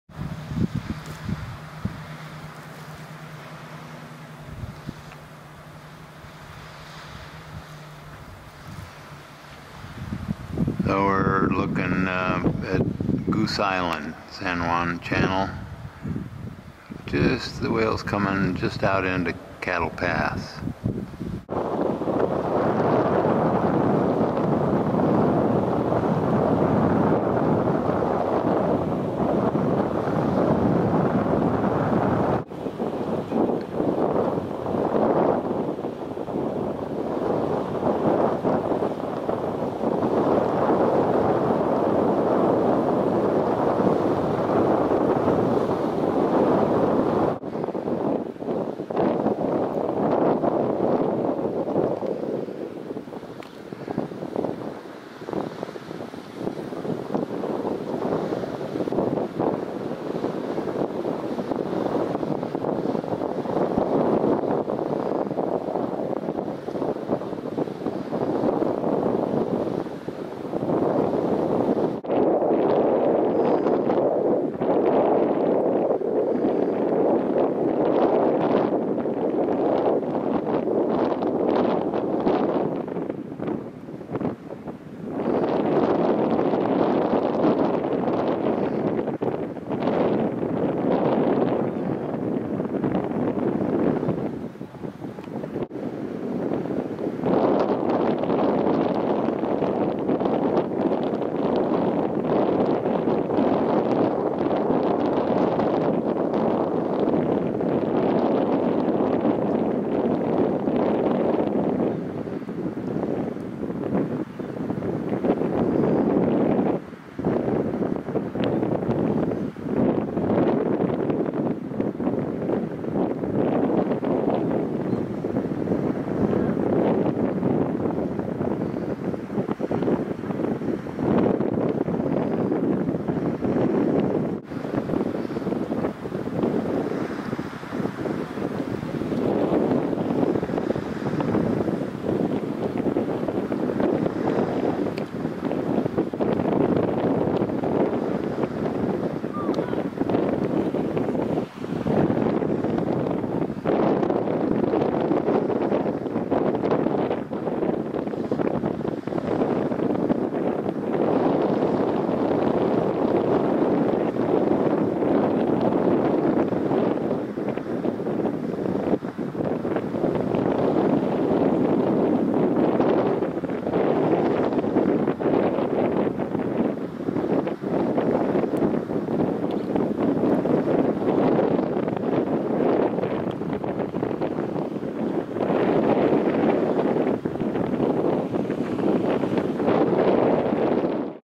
SRKW Today
J,K, and L pods heading through Cattle Pass between San Juan Island and Lopez Island and out into the eastern Strait of Juan de Fuca. 30 November 2013. The splashing activity was probably herding salmon out into open water where they can be circled and herded.